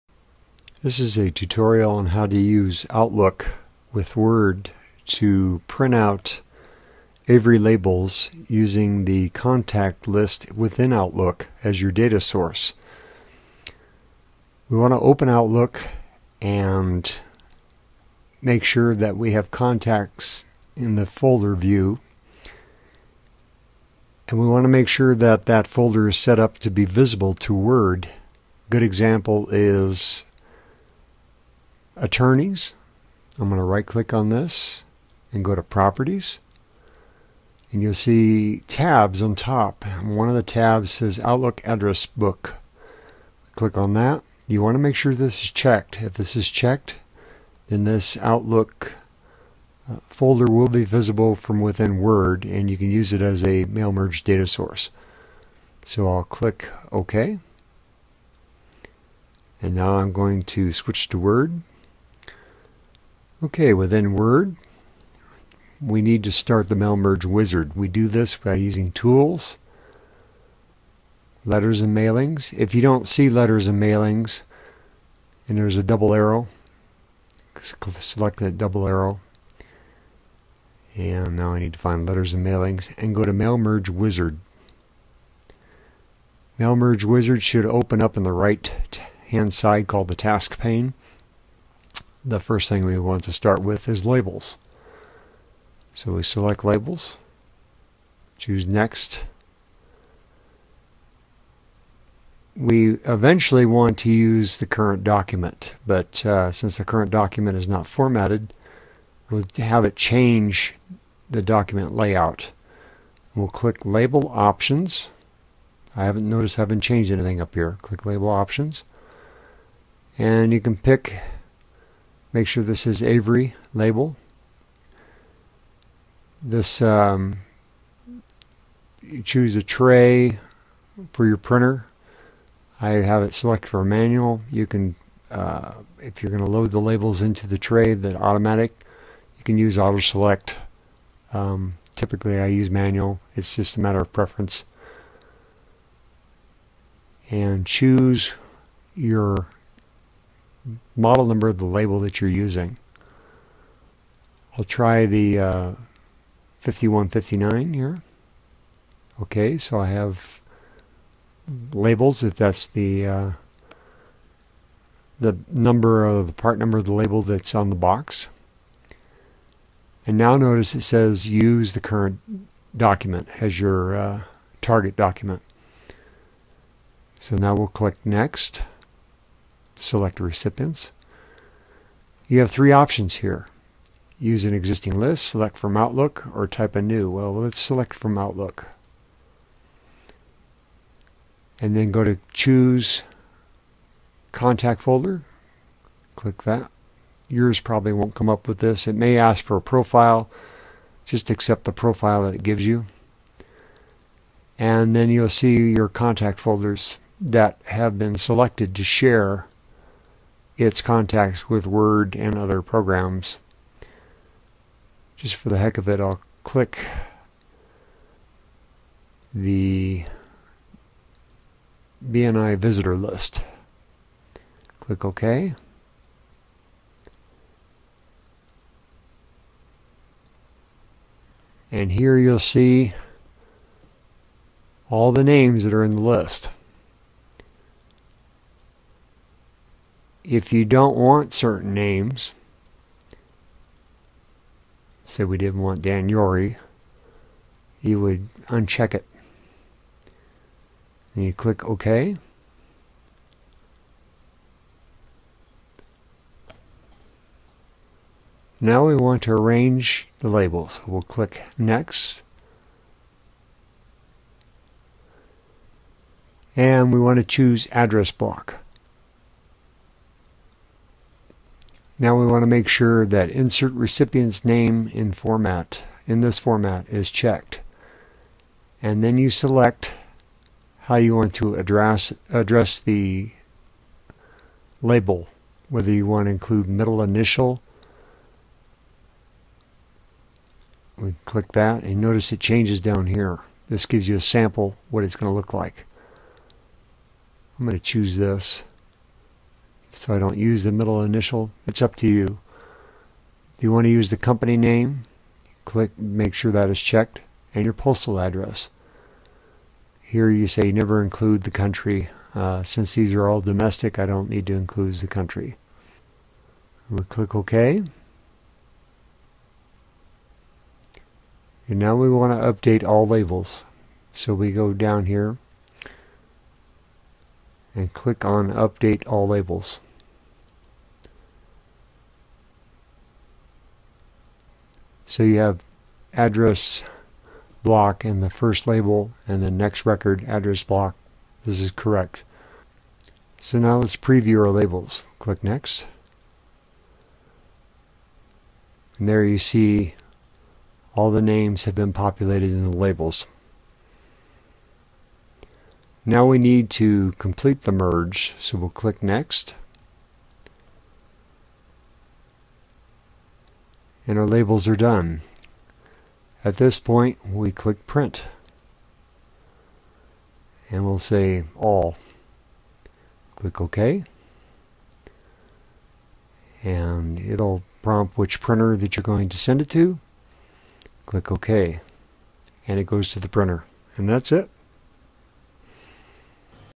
How to use Word 2002/2003 Mailmerge Wizard to print Avery Labels using Outlook Contacts
One of the most requested training sessions by clients is how to use the MailMerge feature in Word to Print Labels, using contacts from Outlook Address Book. to view more videos. Word 2002 (XP) simplified the MailMerge function from previous generations of Word, by performing many of the steps for you. We train our customers to use Outlook as the default address book, which can be accessed by several applications (although Outlook 2007 changed its architecture which prevents applications such as WinFax from accessing its data).